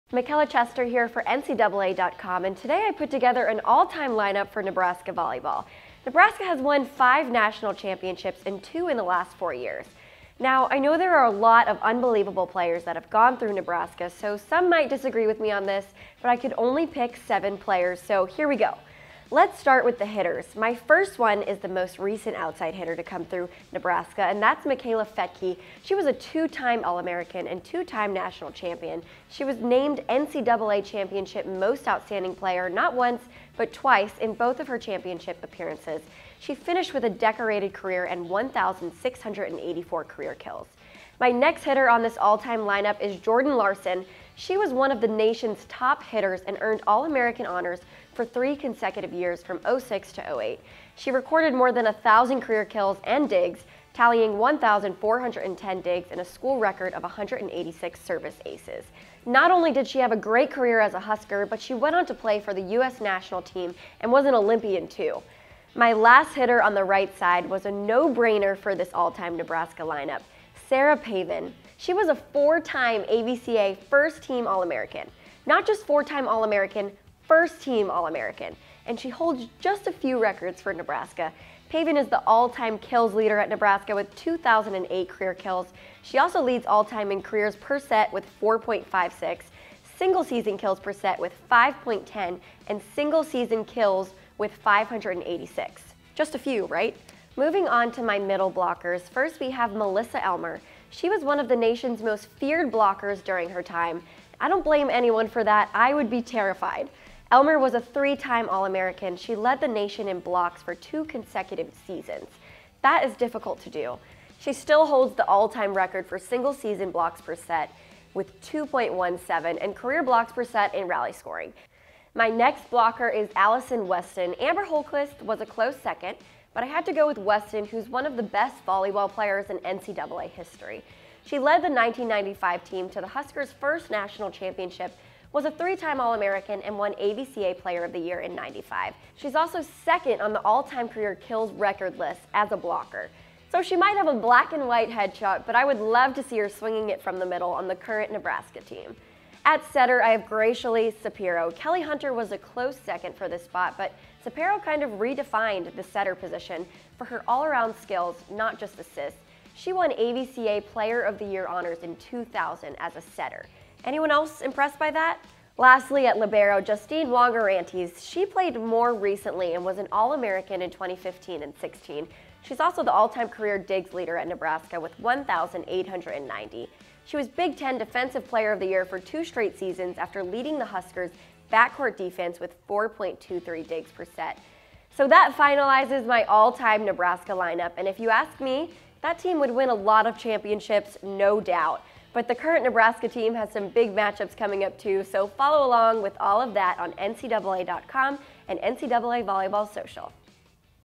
Nebraska volleyball's all-time lineup
NCAA.com's Michella Chester takes a stab at putting together an all-time lineup for Nebraska volleyball. This isn't easy - agree or disagree below in comments.